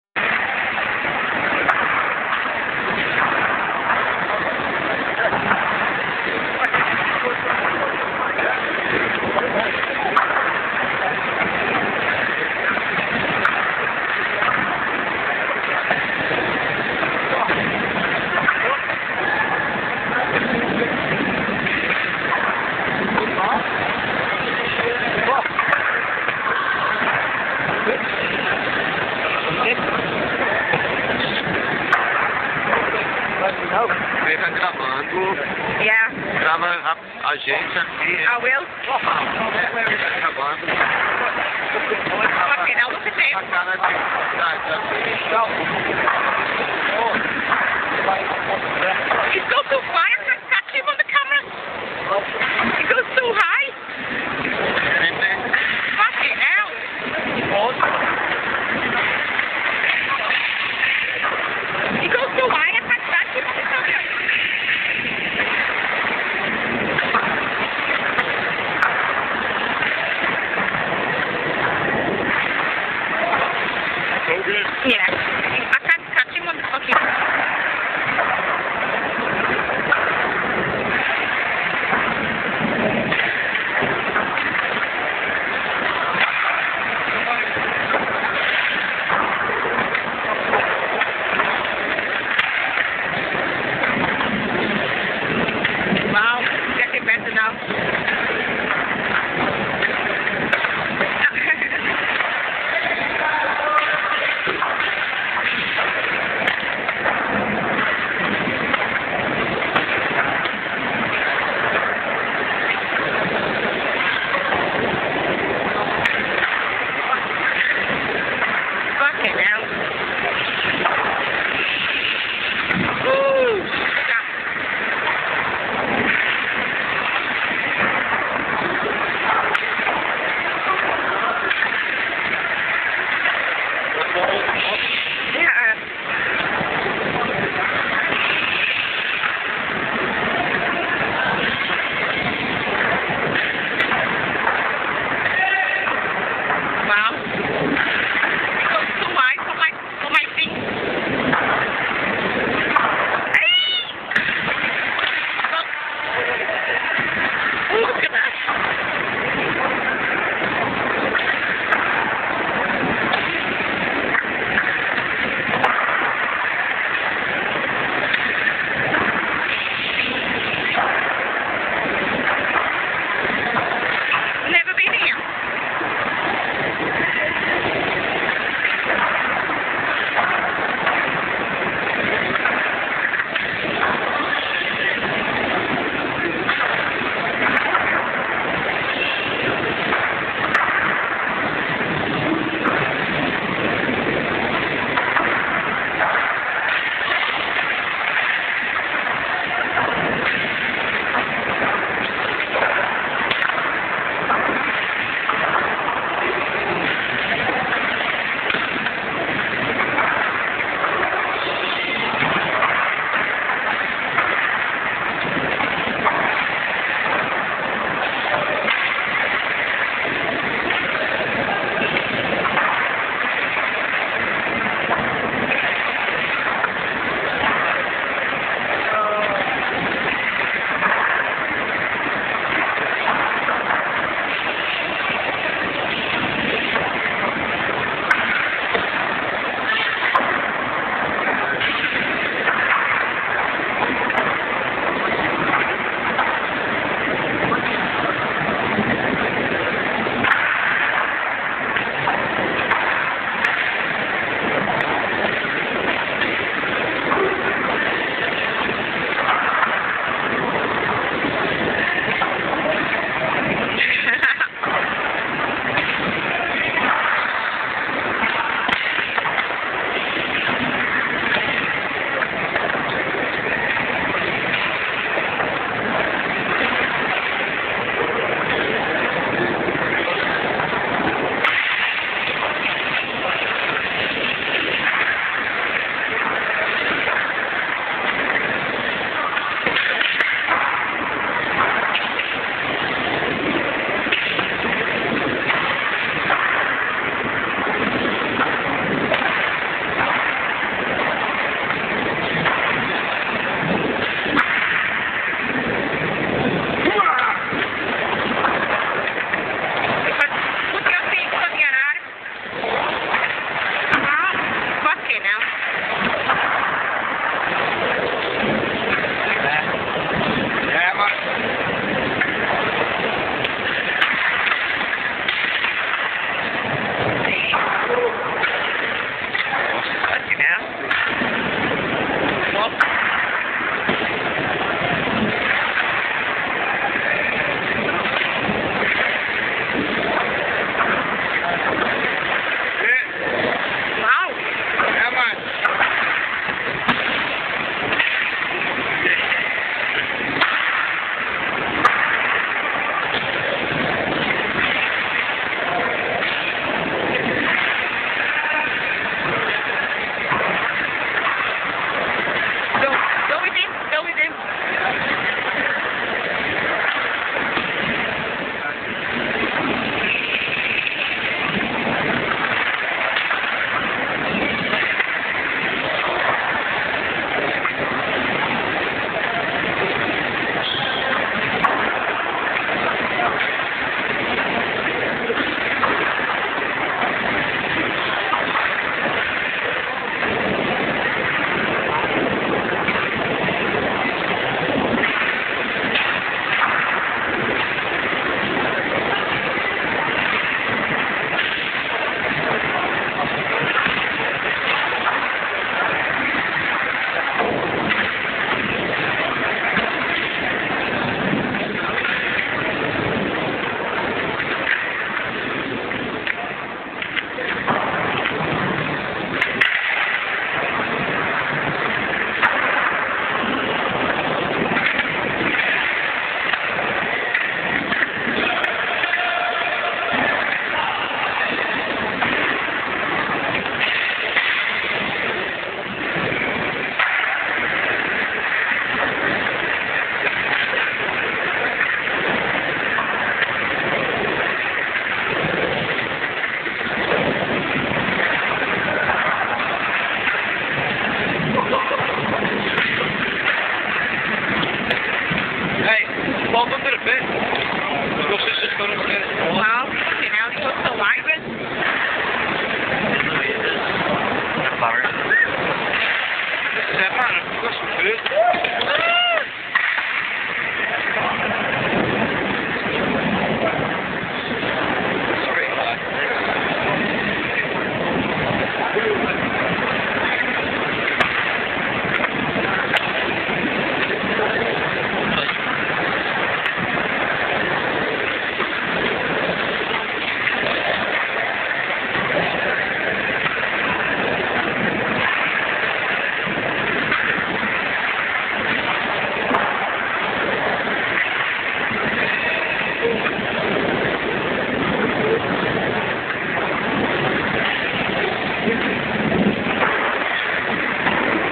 full pipe session
this is thelast time we got to sk8 the full pipe before the minge bag mutha fuckers took it away